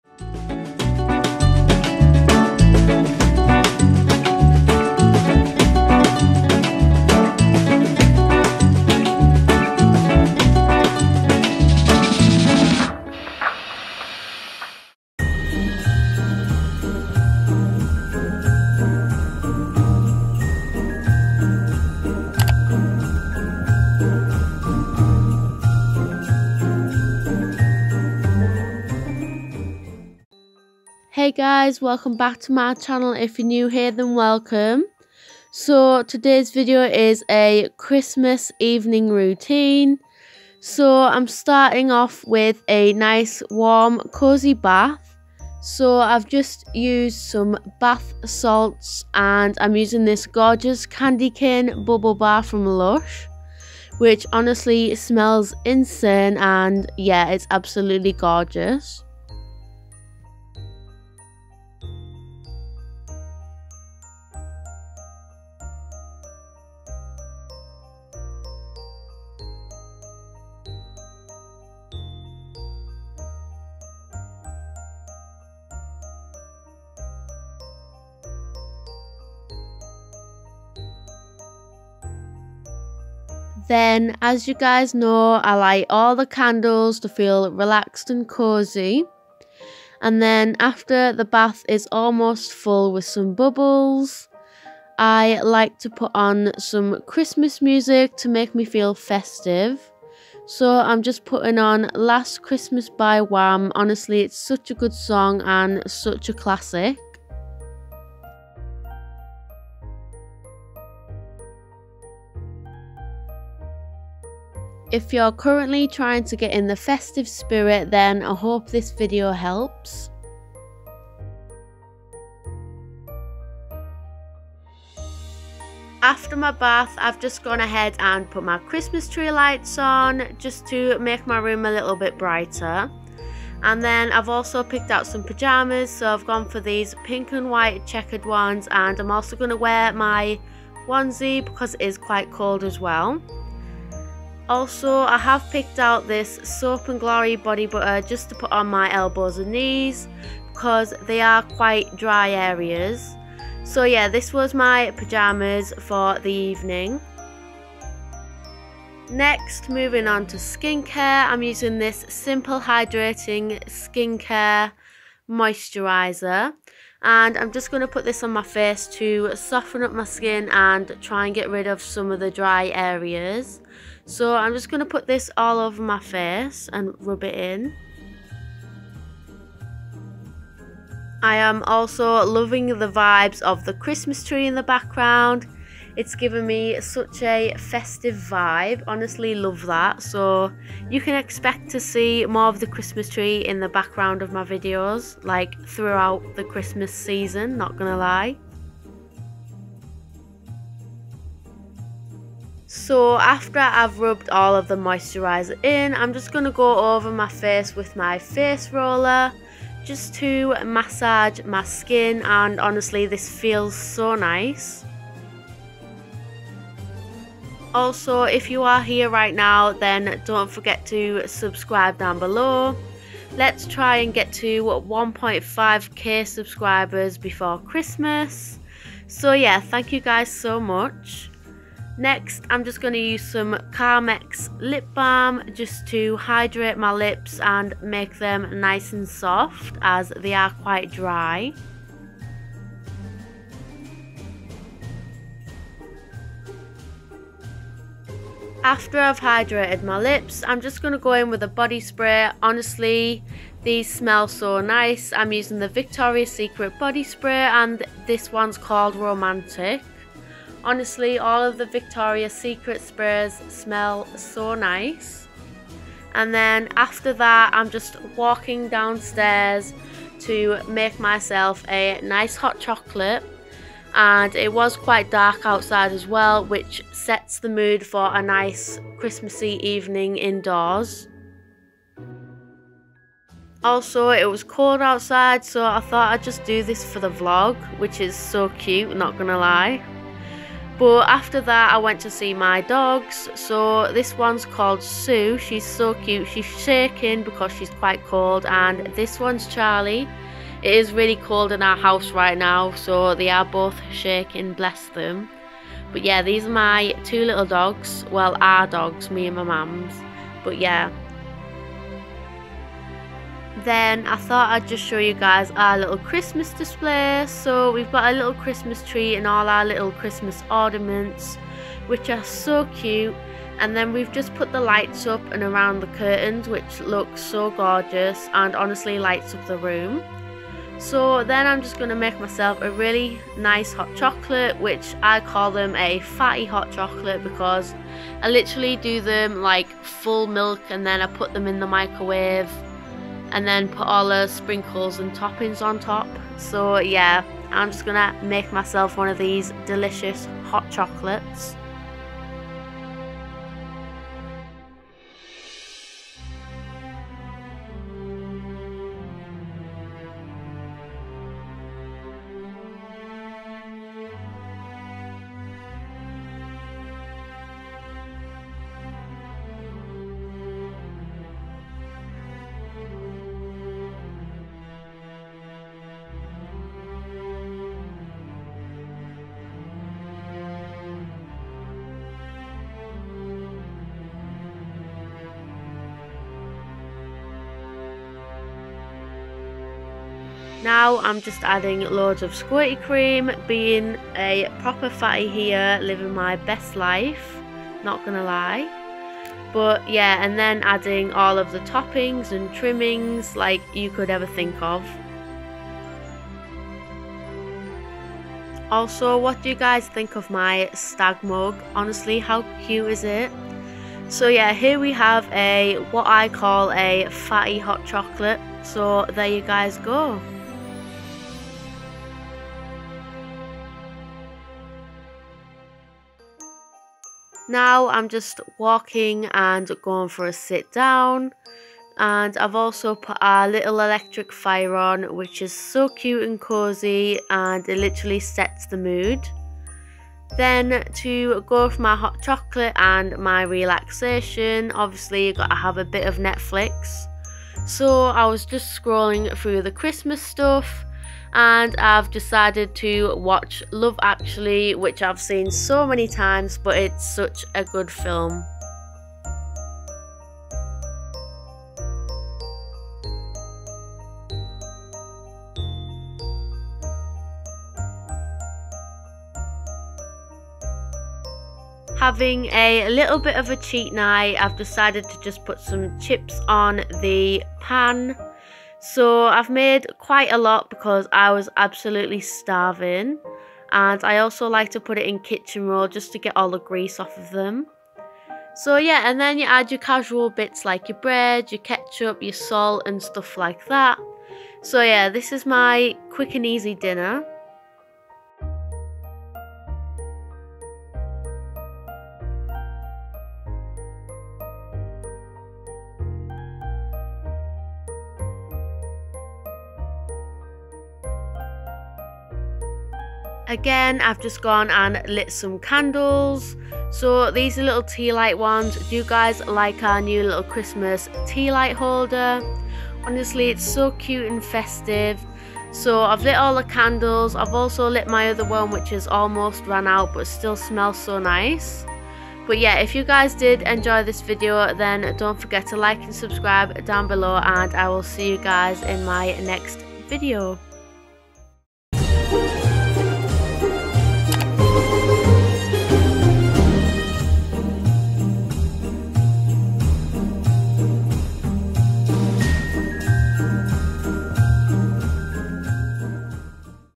CHRISTMAS/HOLIDAY EVENING ROUTINE | Relaxing + Cosy 2021 ✨
Welcome back to another video hope you all enjoy as per usual, had such fun making this video. I am doing weekly vlogmas this year which should be fun ❤ with more Christmas videos in between 🎄stay tuned and have a wonderful evening everyone and I'll see you guys Sunday evening @7pm ❤

THANKYOU 👼🙌

Tasha x

🌟INSTAGRAM- tasha_hind or beautybytashahind
🌟21 BUTTONS-tasha_hind